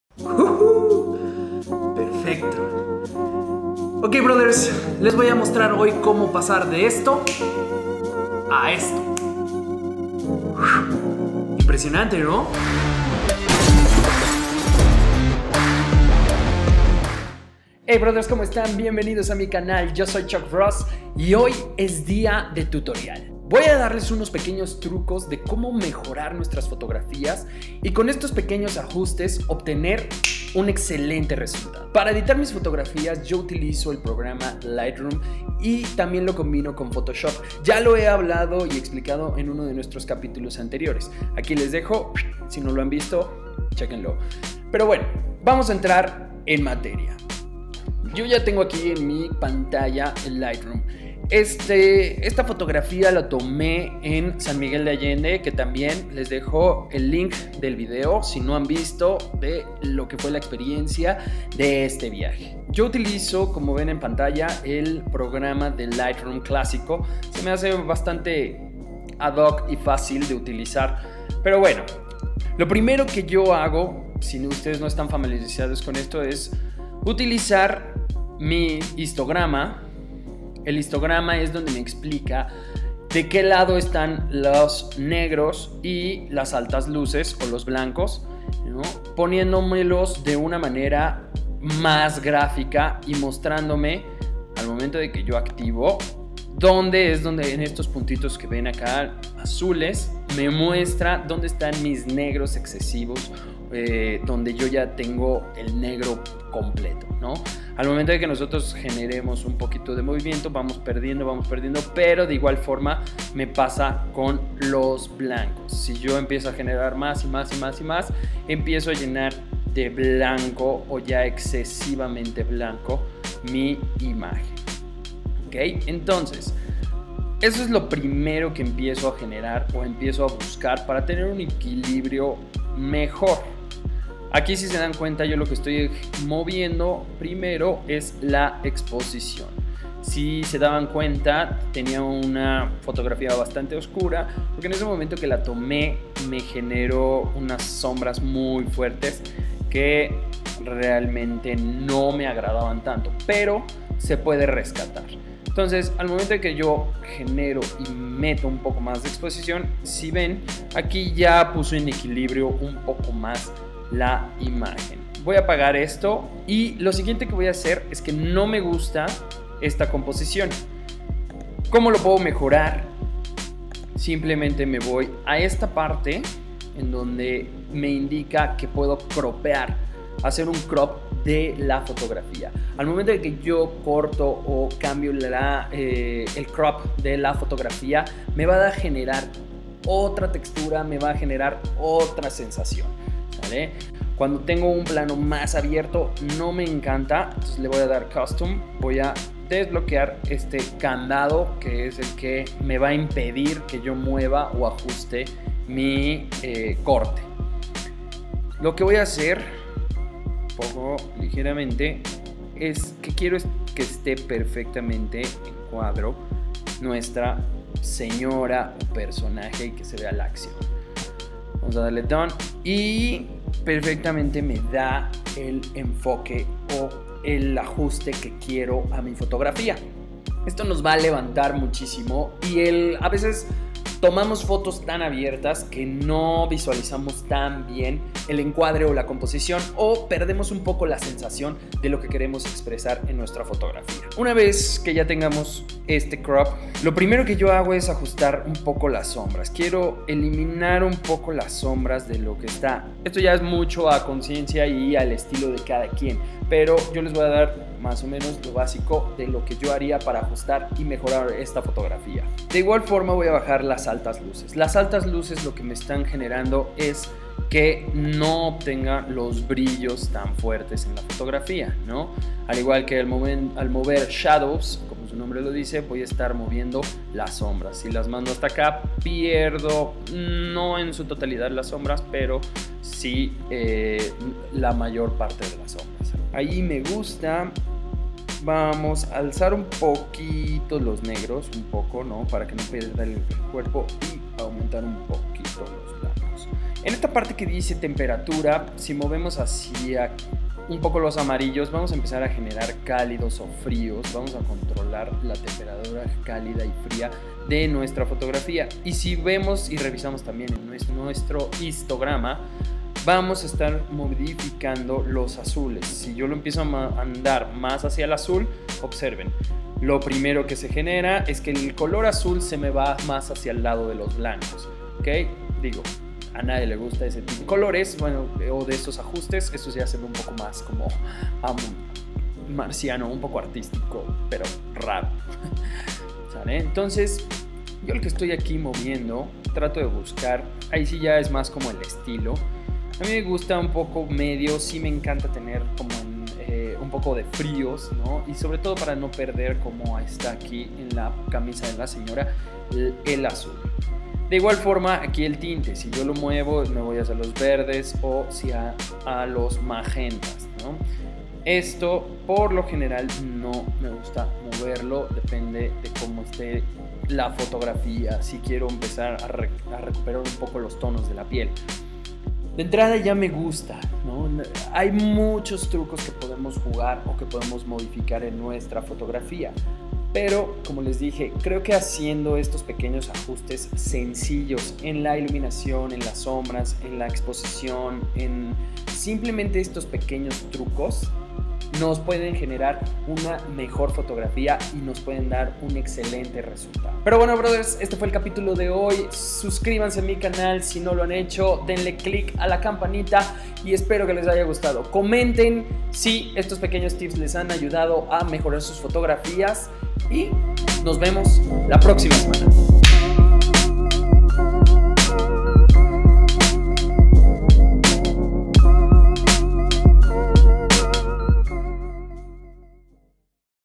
Como editar fotos rápido / Tips para lightroom
El utilizar herramientas para mejorar nuestras fotos es básico, por eso editar una fotografía nos ayuda en obtener una foto mas impresionante.

El programa Lightroom es especial para editar nuestras fotos, por eso les comparto unos pequeños tips que pueden ayudarlos a editar de una forma básica.

La forma de editar de cada quien es importante, ya que de esa forma podemos mejorar nuestro estilo y poder obtener mejores resultados.

En mi experiencia para editar, prefiero siempre comenzar por nivelar la exposición de una fotografía, corrigiendo los niveles en el histograma.

Les comparto unos videos del canal que les puedan interesar.